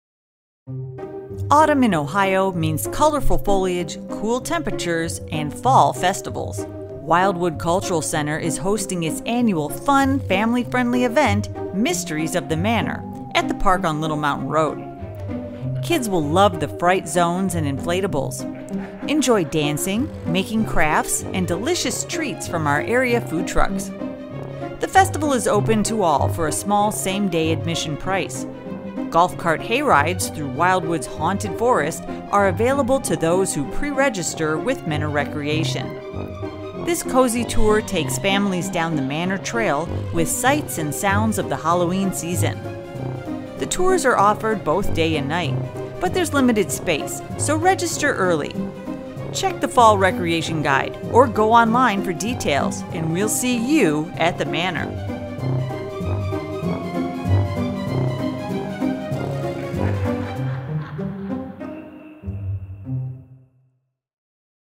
Mysteries of the Manor Trail Fall Festival 2024
Mysteries of the Manor Trail GOLF CART HAYRIDE!
FRIDAY, OCTOBER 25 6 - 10 p.m. & SATURDAY, OCOTOBER 26, 2- 10 p.m.!
Wildwood Cultural Center, 7645 Little Mountain Road, Mentor
Celebrate fall at Wildwood’s one-of-a-kind GOLF CART HAYRIDE! Cozy up with your family in a golf cart as you view the sights and sounds of the Mysteries of the Manor Trail!  Price per ticket includes:,
-Haunted Hayride Golf Cart Ride through the woods of the Wildwood Estate
-Kids ages one and older can enjoy the fun and entertainment of the Kid's Game Zone
-Dancing/Music DJ Entertainment
-Shop from over two dozen vendors located both inside the Wildwood Estate and the haunted grounds
-Enjoy Face Painting and Character Artist (for Purchase)
-Enjoy food for Purchase from local food trucks and eateries
-Hilarious and frightful Halloween jokes and so much more!
More active scaring times are  6-10pm.
Children under 1 are FREE.
Mentor Resident: $18 / Nonresident: $20